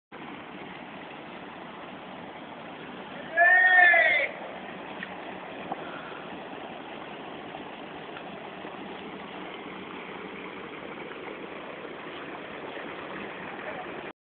Espuma en la fuente
Durante la huelga de parques y jardines alguien echó "algo" en la fuente con el consiguiente resultado.